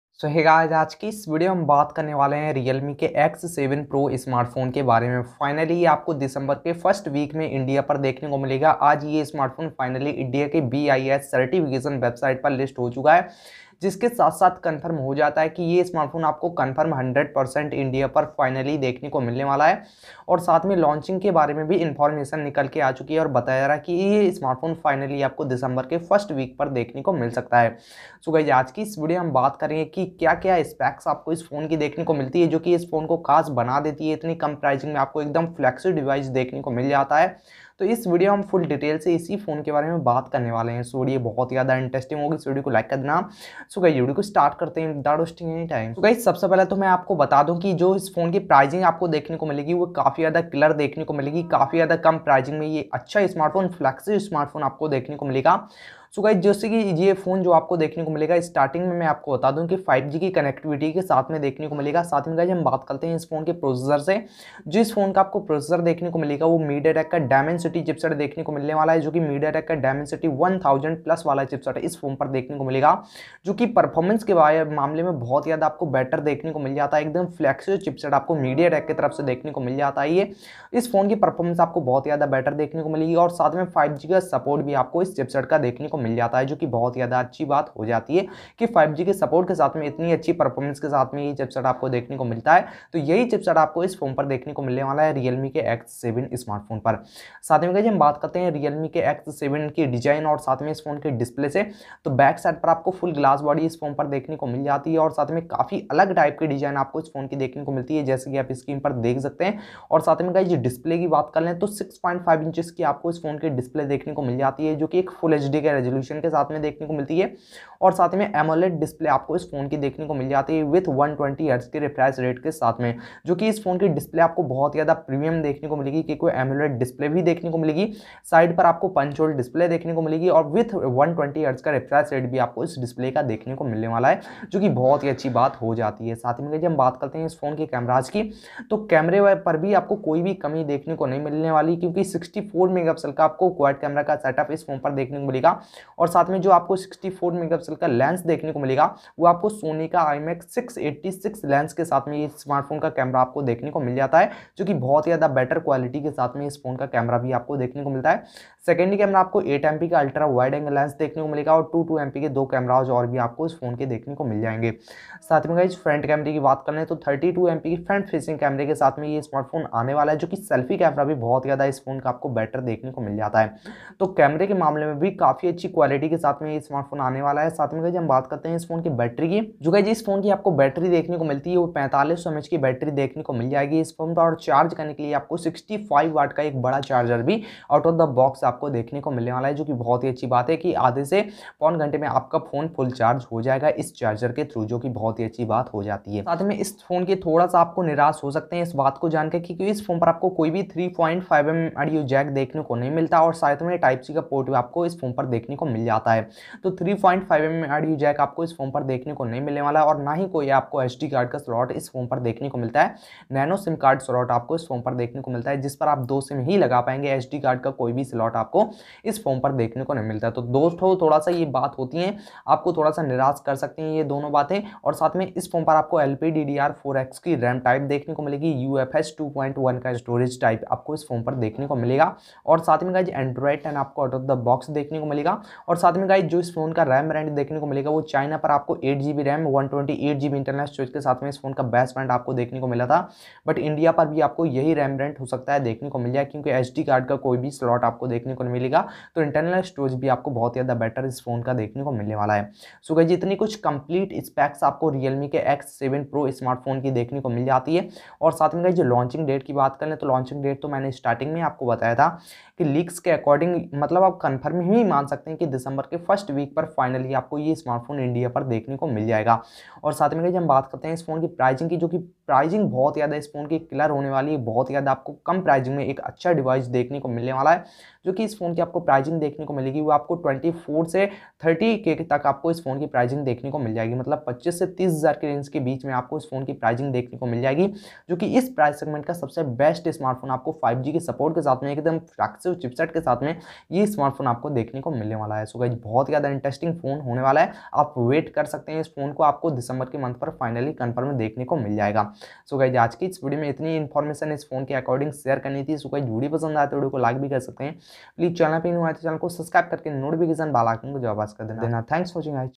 Realme X7 Pro India Launch Date Confirmed | Price In India | Specifications | Realme X7 Series
So guys ajj ki is video me apko btane wala hu realme ke x7 Series k bare m realme x7 pro price, realme x7 pro launch date, realme x7 pro specification and all information realme x7 in this video.

Cover Topic.

Realme X7 pro Series
Realme X7 pro price
Realme X7 pro launch date
Realme X7 pro Specifications
Realme x7 pro review